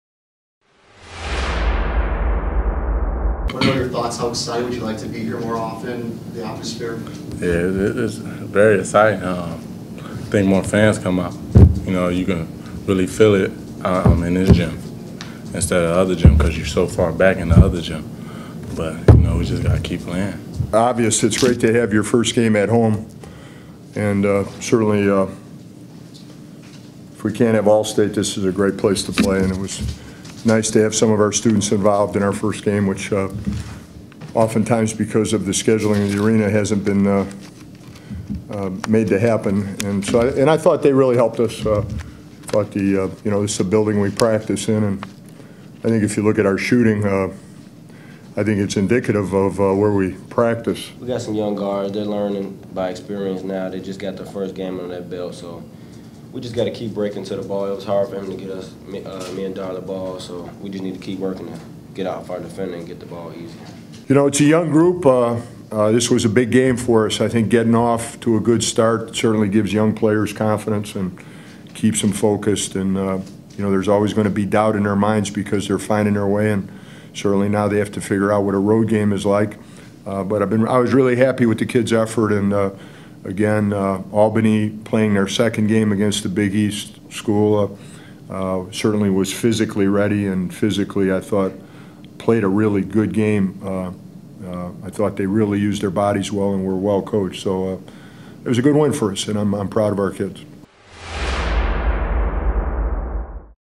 DePaul Takes A Bite Out Of The Great Danes
Dar Tucker scored a game-high 23 points as the Blue Demons opened the 2008-09 campaign with a 75-64 win over Albany on Monday at McGrath Arena. MOUTHPIECESPORTS has all the postgame reaction.